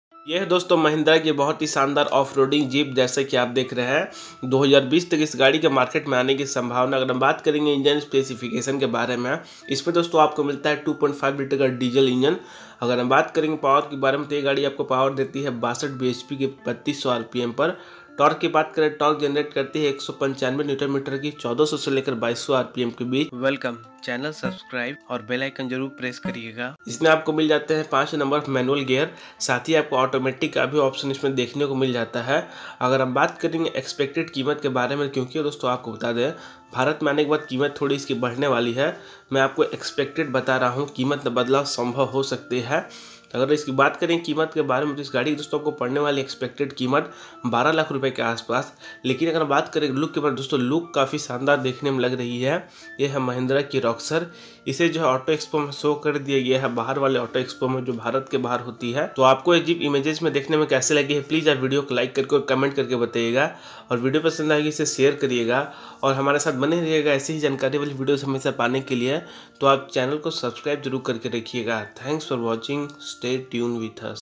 2020 Mahindra Roxor Upcoming in india,Price in India, Images,Engine Specification Hindi Review
This video about..2020 Mahindra Roxor havinf Engine: 2.5 L M2DICR turbo (diesel) with Transmission: 5-speed manual & 2-speed low ratio manual & transfer case 6 speed automatic and expected price in india is Rs. 12,00,000.

Queries Solved:
1.)2020 Mahindra Roxor Price & engine Specs
2.)New Upcoming Mahindra SUV 2020 Mahindra Roxor
3.)Price in india 2020 Mahindra Roxor